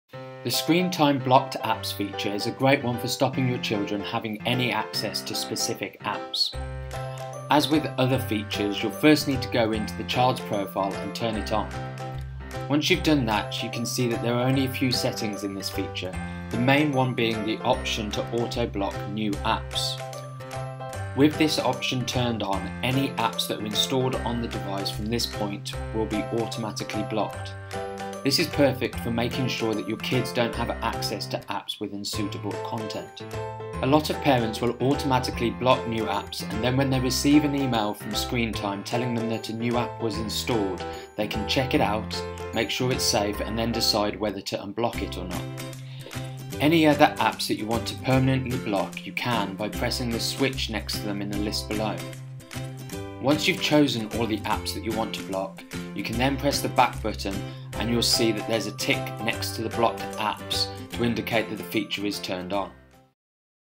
Screen Time - Blocked Apps
Want to prevent your children from accessing certain apps at any time? Then try out the Blocked Apps feature of Screen Time.